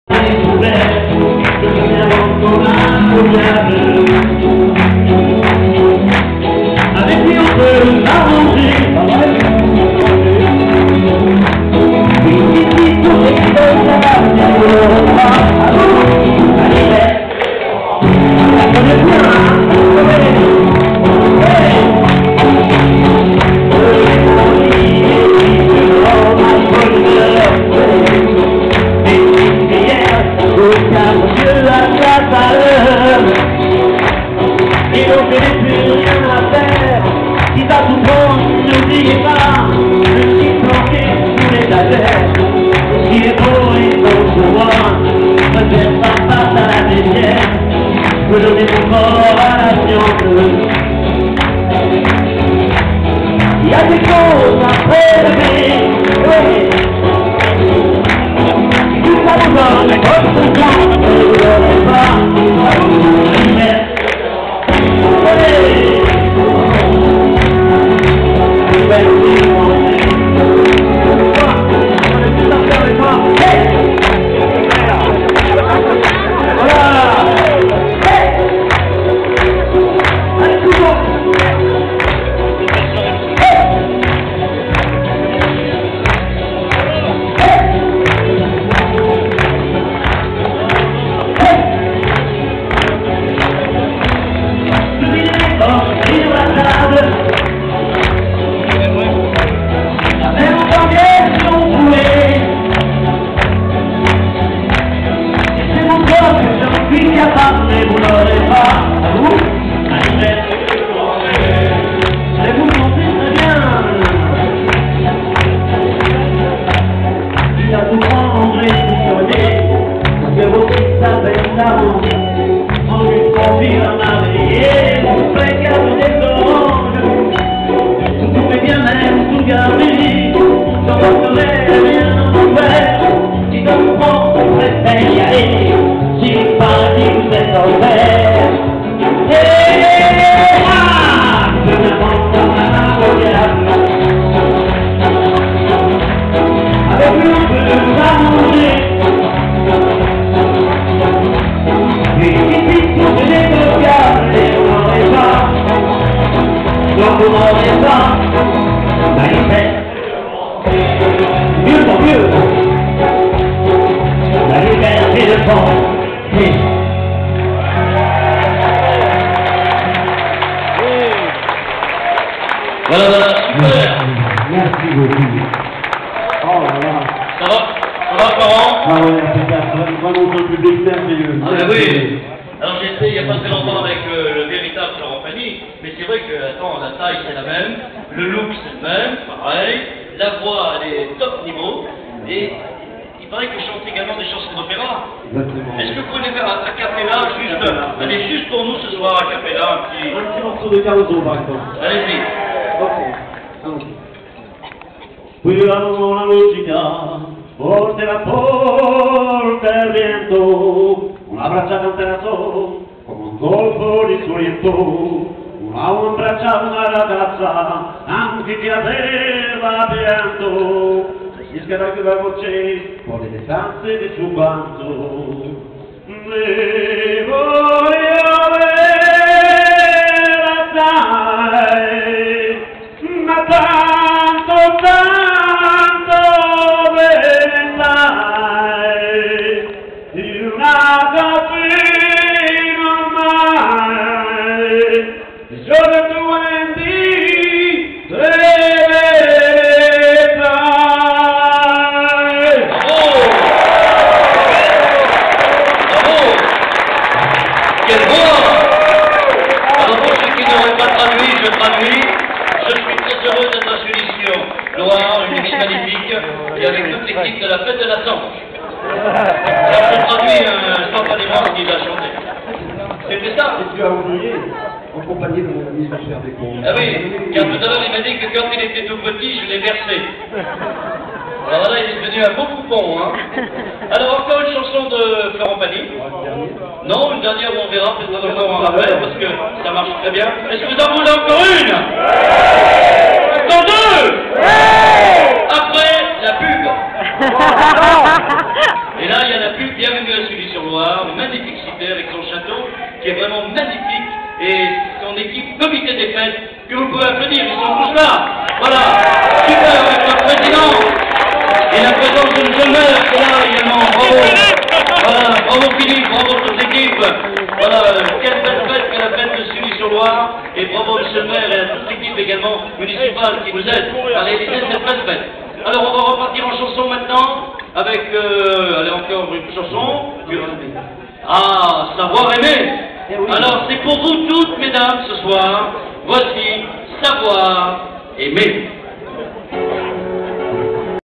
FLORENT DADY ,JEAN PIERRE DESCOMBES .video Djemy.
Florent Dady gala a Sully Sur Loire ,avec JEAN PIERRE DESCOMBES ..